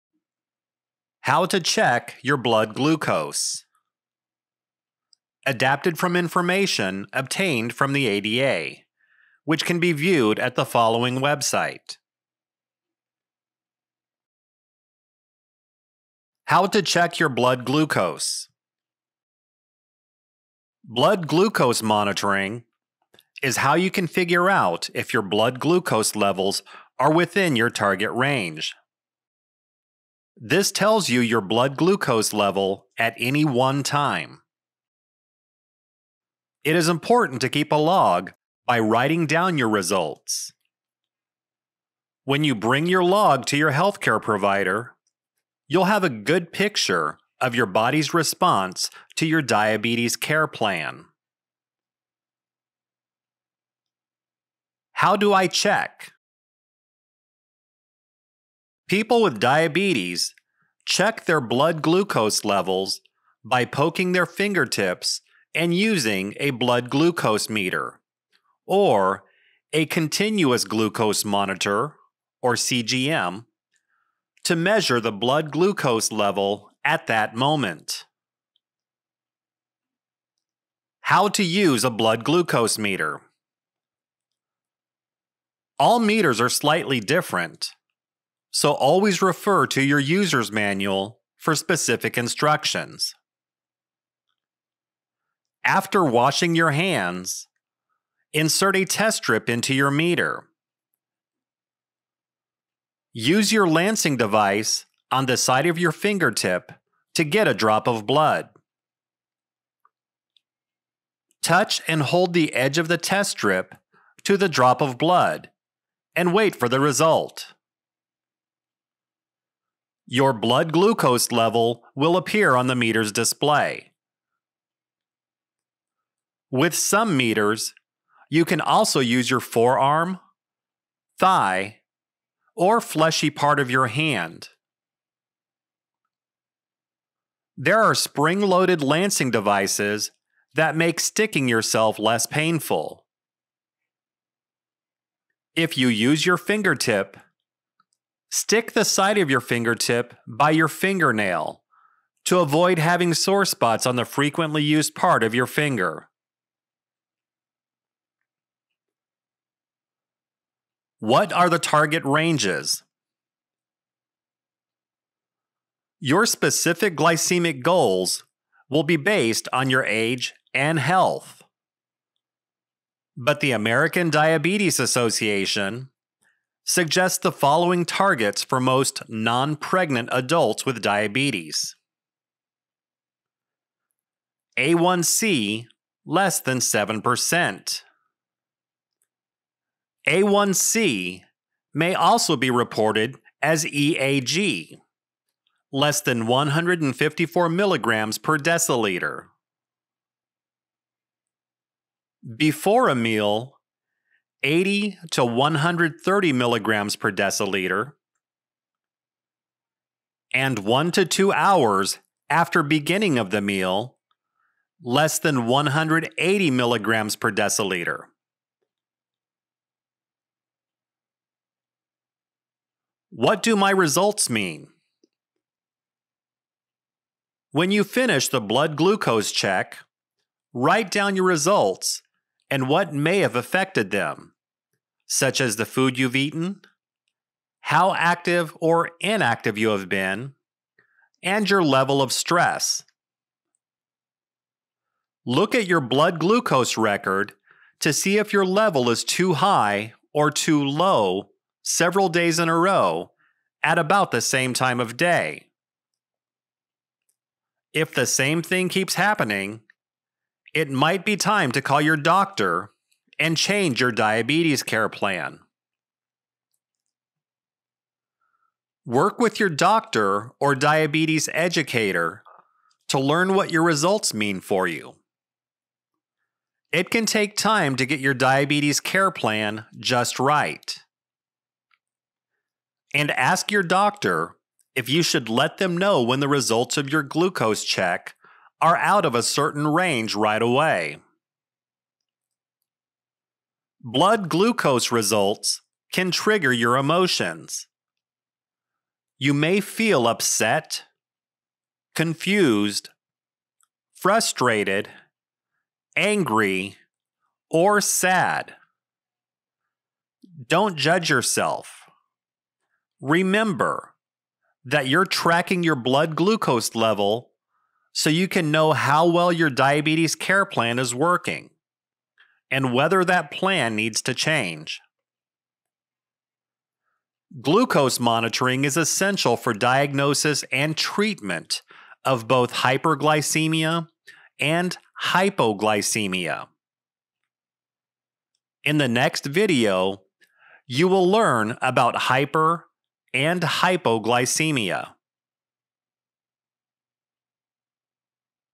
Blood Glucose
These videos were developed for research purposes and are the first iteration of an American Sign Langauge-Accessible Diabetes Education Intervention.  Video production, ASL, and captions were provided by project partner Deaf Link, Inc.